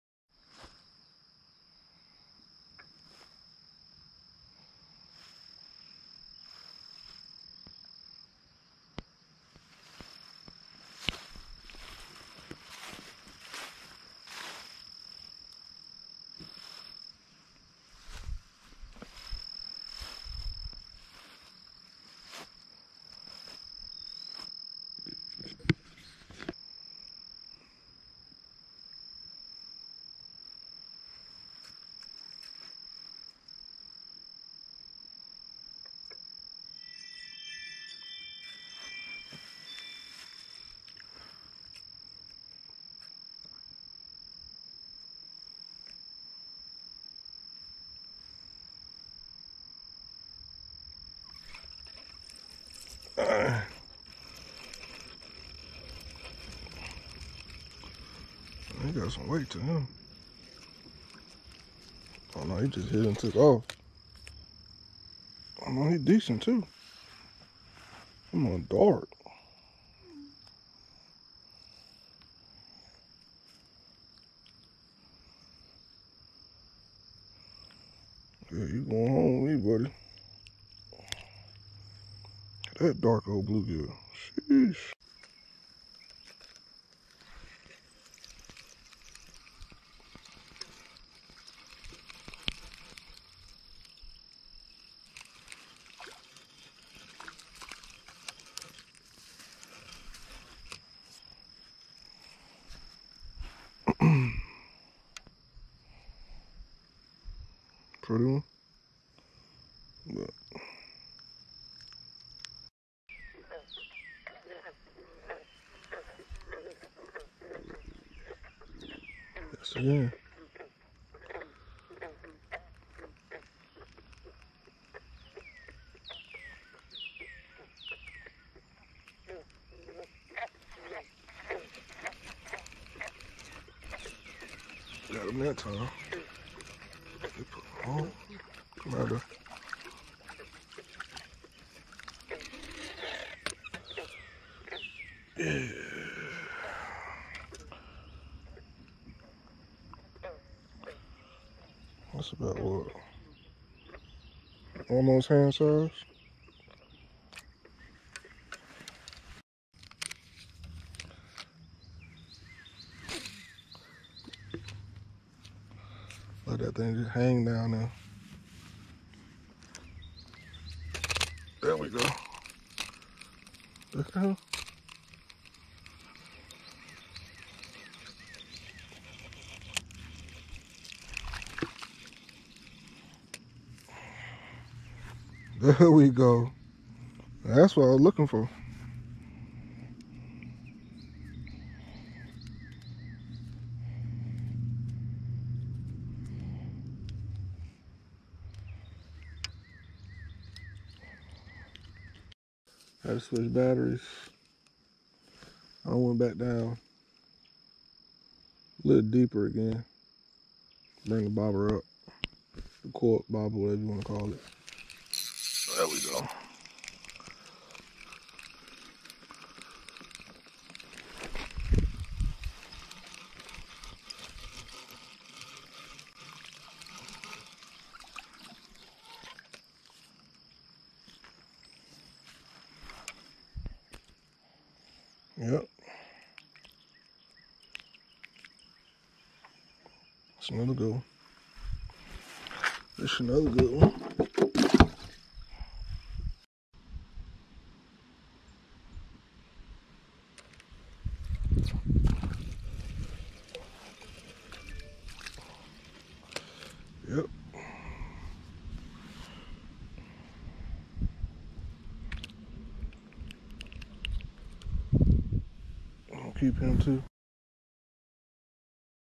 Fishing fishing pondfishing bluegillfishing
Late evening fishing at the pond with worms and a bobber🎣